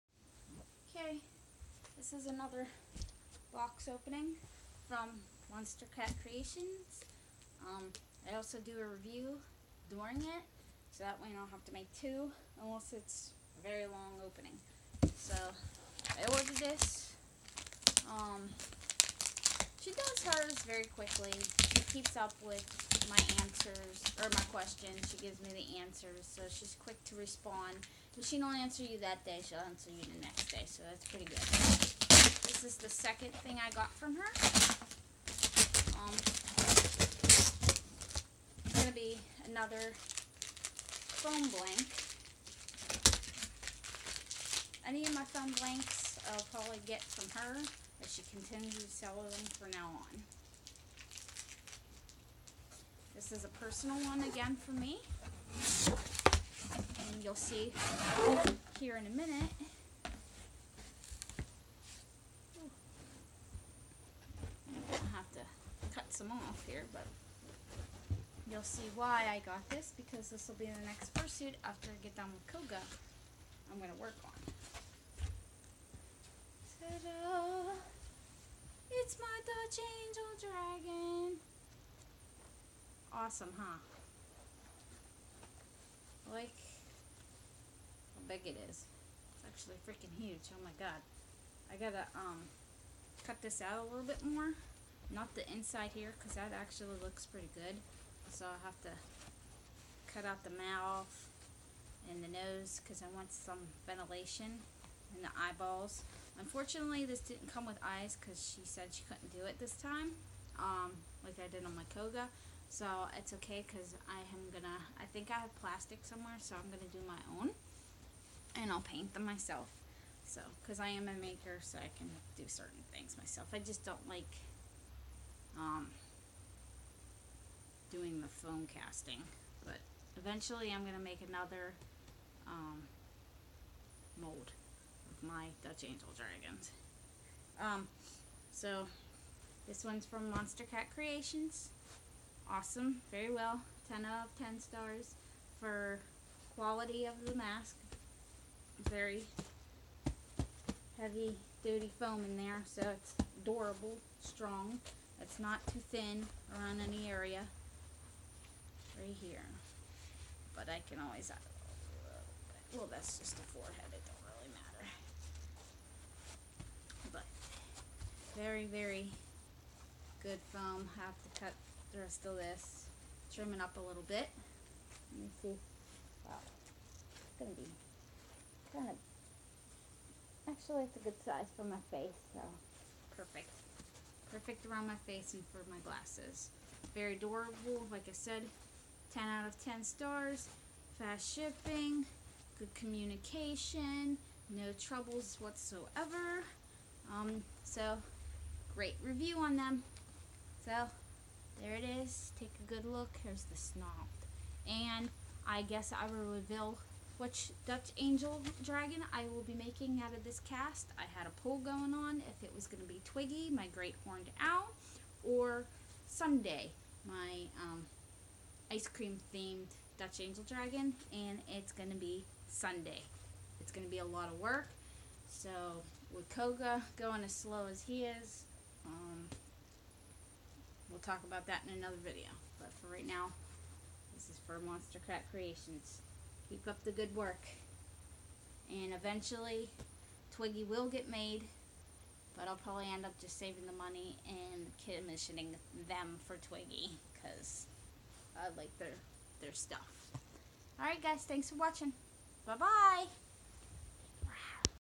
Unboxing video of foam blank
Got another foam blank from Monstercat creations. They are great 10 out of 10.